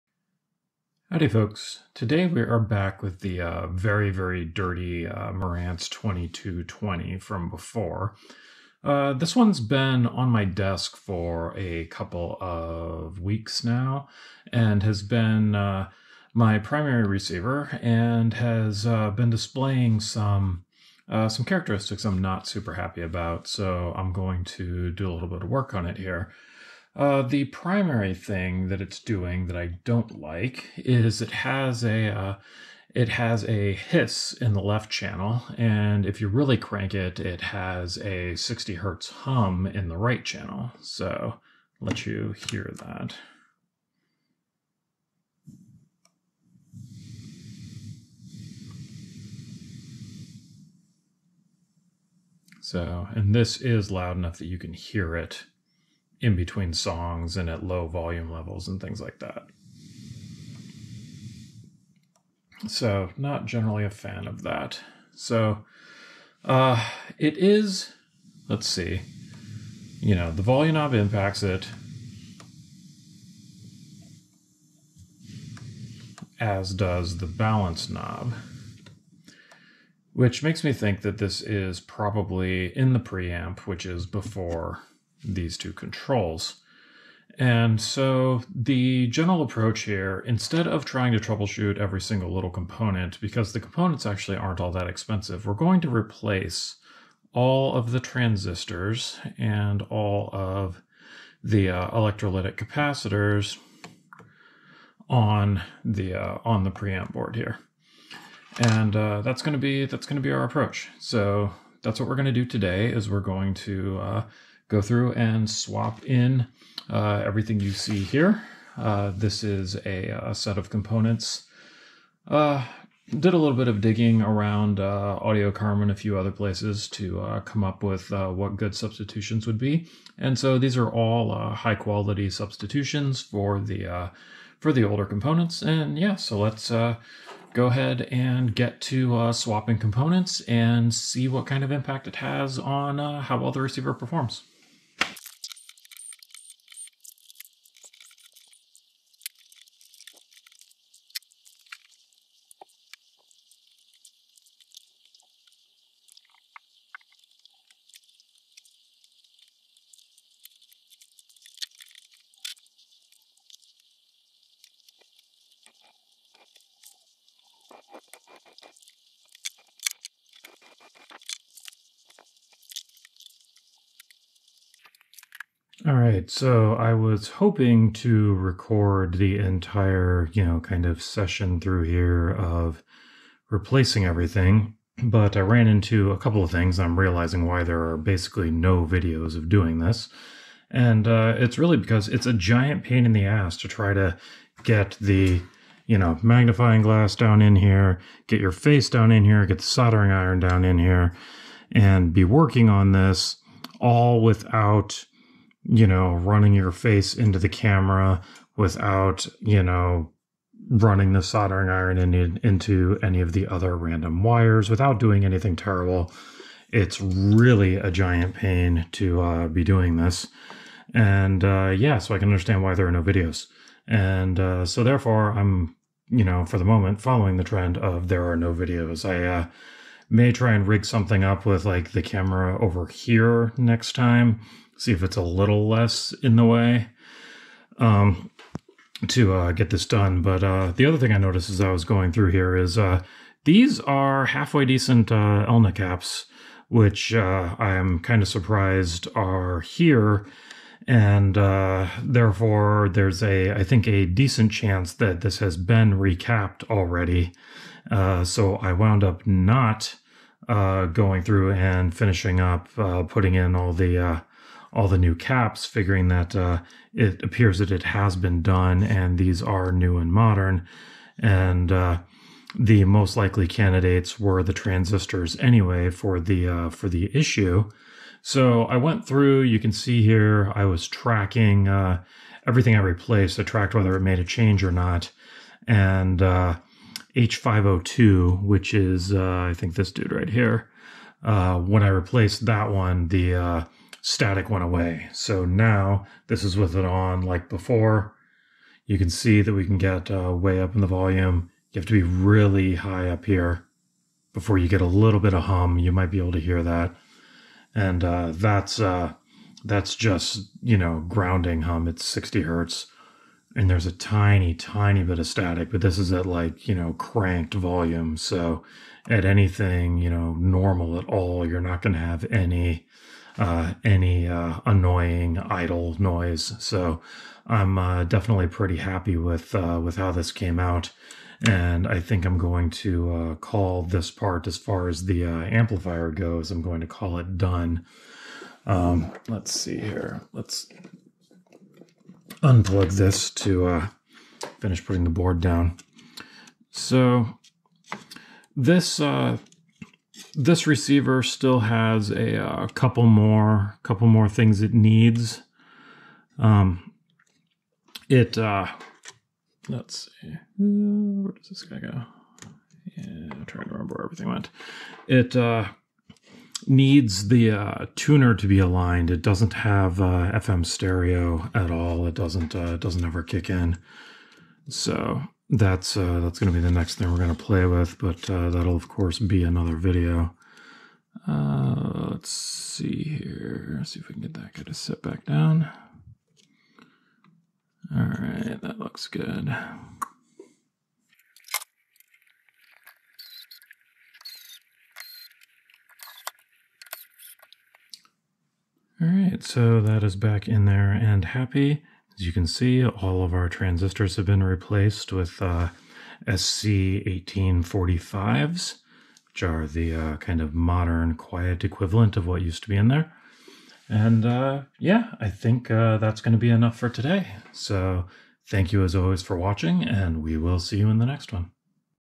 Marantz 2220: Replacing Transistors to Reduce Hiss
If you're interested in any of the equipment I work on, most of it winds up on eBay to fund future projects

00:00 Introduction
00:26 Hiss Demonstration
02:25 Futzing With the Pre-Amp
02:55 Videos are Hard
04:37 Post Swap Testing

Useful products from the video:
• Hakko FX888D Soldering Station
• Precision Electric Screwdriver